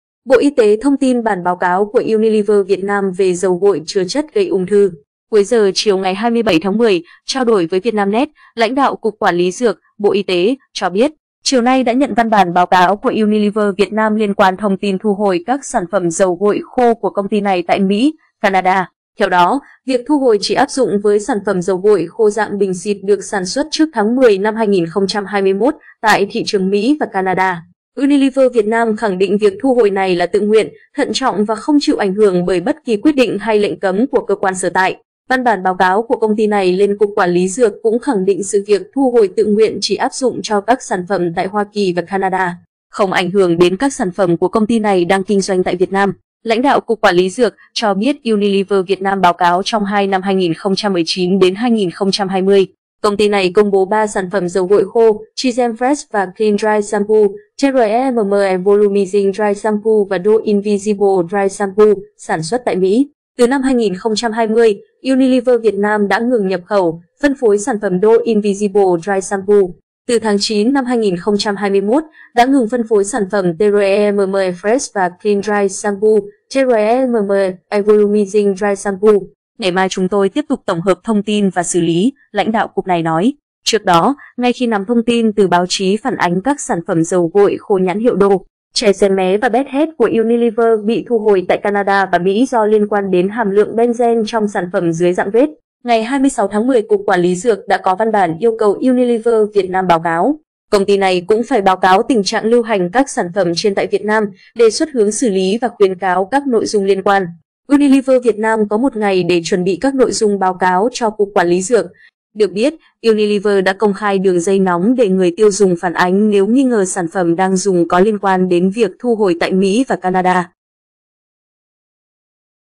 Bộ Y tế thông tin bản báo cáo của Unilever Việt Nam về dầu gội chứa chất gây ung thư
Cuối giờ chiều 27/10, trao đổi với VietNamNet, lãnh đạo Cục Quản lý dược (Bộ Y tế) cho biết, chiều nay đã nhận văn bản báo cáo của Unilever Việt Nam liên quan thông tin thu hồi các sản phẩm dầu gội khô của công ty này tại Mỹ, Canada.Lãnh đạo Cục Quản lý dược cho biết Unilever Việt Nam báo cáo trong 2 năm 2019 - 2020, công ty này công bố 3 sản phẩm dầu gội khô (TRESemme Fresh &amp; Clean Dry Shampoo; TRESemme Volumizing Dry Shampoo và Dove Invisible Dry Shampoo) sản xuất tại Mỹ.
Công ty này cũng phải báo cáo tình trạng lưu hành các sản phẩm trên tại Việt Nam, đề xuất hướng xử lý và khuyến cáo các nội dung liên quan. Unilever Việt Nam có 1 ngày để chuẩn bị các nội dung báo cáo cho Cục Quản lý dược.
Được biết, Unilever đã công khai đường dây nóng để người tiêu dùng phản ánh nếu nghi ngờ sản phẩm đang dùng có liên quan đến việc thu hồi tại Mỹ và Canada.
Unilever Việt Nam khẳng định việc thu hồi này là “tự nguyện”, “thận trọng” và không chịu ảnh hưởng bởi bất kỳ quyết định hay lệnh cấm của cơ quan sở tại.
“Ngày mai chúng tôi tiếp tục tổng hợp thông tin và xử lý” – lãnh đạo Cục này nói.
Từ năm 2020, Unilever Việt Nam đã ngừng nhập khẩu, phân phối sản phẩm Dove Invisible Dry Shampoo. Từ tháng 9/2021 đã ngừng phân phối sản phẩm TRESemme Fresh &amp; Clean Dry Shampoo, TRESemme Volumizing Dry Shampoo.
Văn bản báo cáo của công ty này lên Cục Quản lý dược cũng khẳng định sự việc thu hồi tự nguyện chỉ áp dụng cho các sản phẩm tại Hoa Kỳ và Canada, không ảnh hưởng đến các sản phẩm của công ty này đang kinh doanh tại Việt Nam.
Trước đó, ngay khi nắm thông tin từ báo chí phản ánh các sản phẩm dầu gội khô nhãn hiệu Dove, TRESemmé và Bed Head của Unilever bị thu hồi tại Canada và Mỹ do liên quan đến hàm lượng benzene trong sản phẩm dưới dạng vết, ngày 26/10 Cục Quản lý dược đã có văn bản yêu cầu Unilever Việt Nam báo cáo.
Theo đó, việc thu hồi chỉ áp dụng với sản phẩm dầu gội khô dạng bình xịt được sản xuất trước tháng 10/2021 tại thị trường Mỹ và Canada.